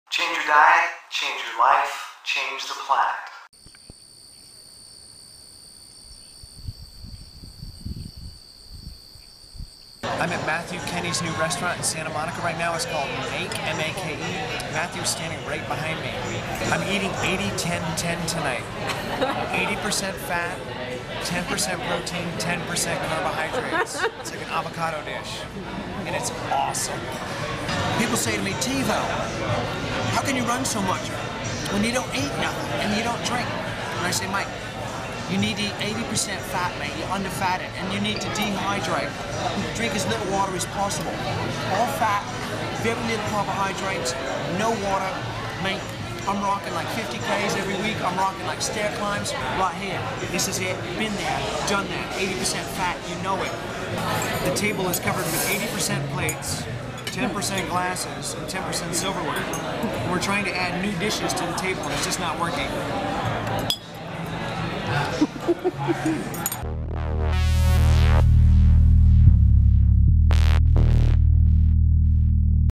Tim Van Orden eats 80/10/10
How to lose weight, get fit, succeed on a diet, deal with obstacles and change your life.
Welcome to Tim Van Orden's Running Raw Project. In this episode: Tim has a little fun while at Matthew Kenney's new restaurant M.A.K.E. in Santa Monica, CA. It seems that Matthew has taken the 80/10/10 diet into consideration when he prepared the menu.